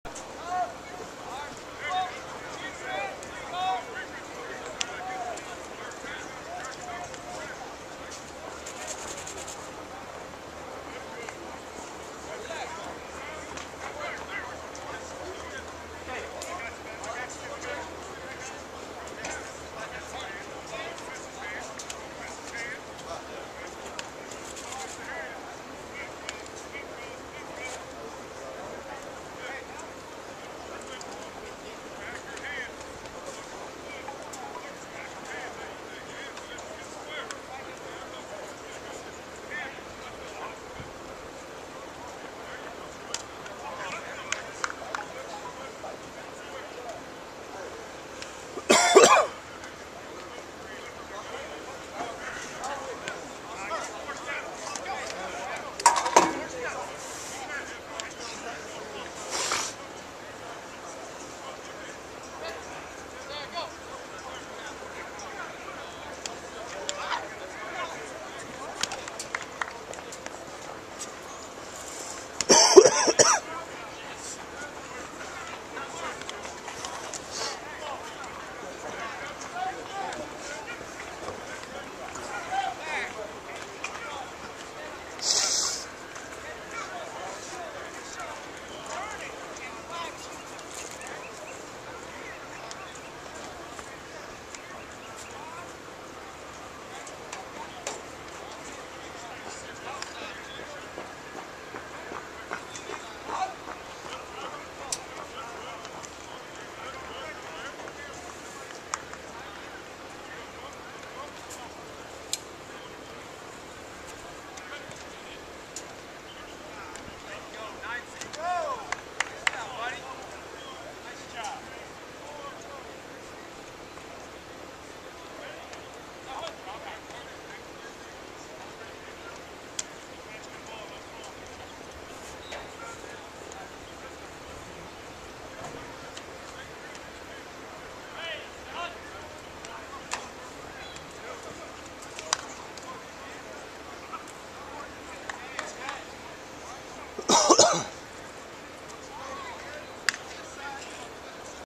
2016 Appalachian State Mountaineers @ Greenville Football Camp June 13
Clayton Comets rising junior QB Landen Leiser stands about 6'3" and weighs 185 pounds.  He is participating in approximately a dozen camps during the summer.  Clayton Comets teammates Noah Williams & Austin Cagle also attend the event.